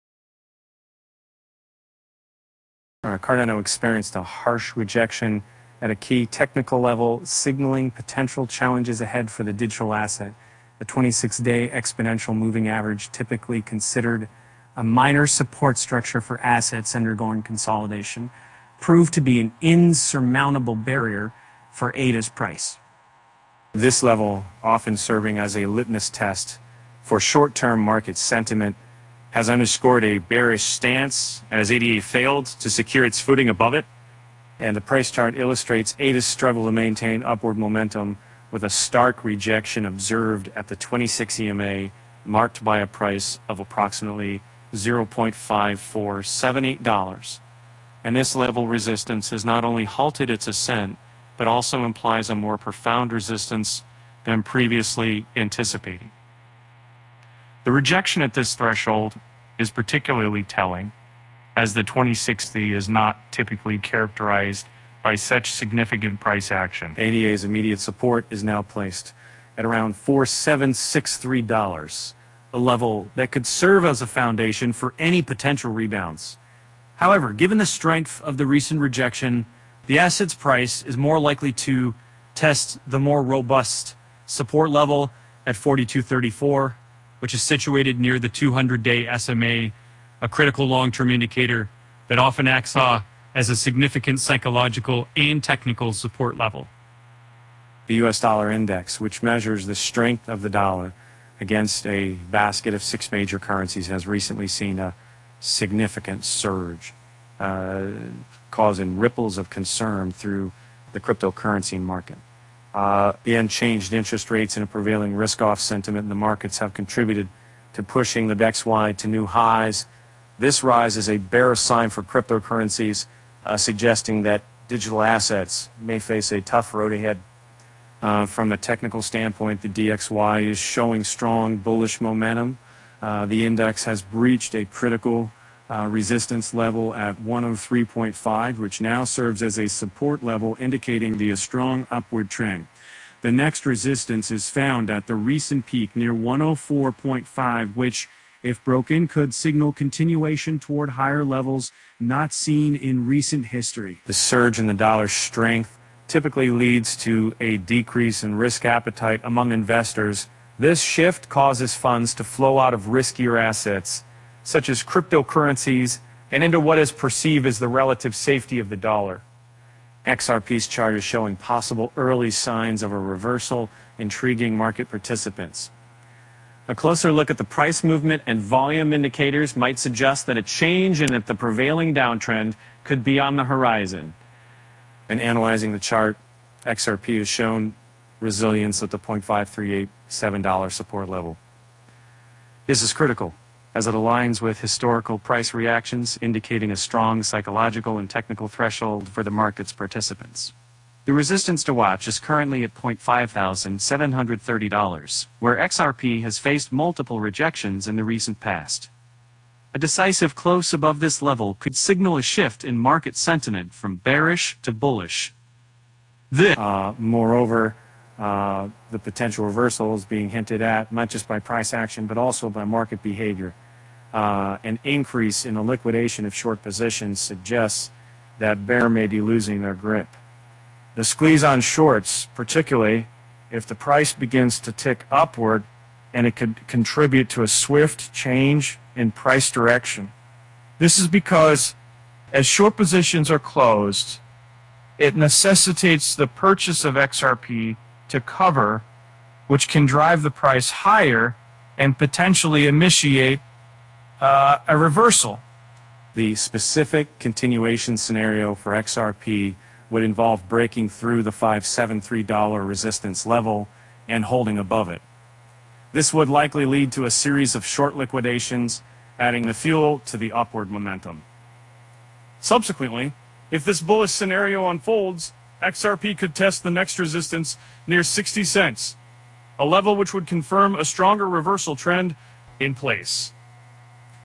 Cardano (ADA) Brutally Rejected, DXY Push to Kill Crypto Market's Momentum, Is XRP Reversal Starting
Cardano experienced a harsh rejection at a key technical level, signaling potential challenges ahead for the digital asset. The 26-day exponential moving average, typically considered a minor support structure for assets undergoing consolidation, proved to be an insurmountable barrier for ADA's price.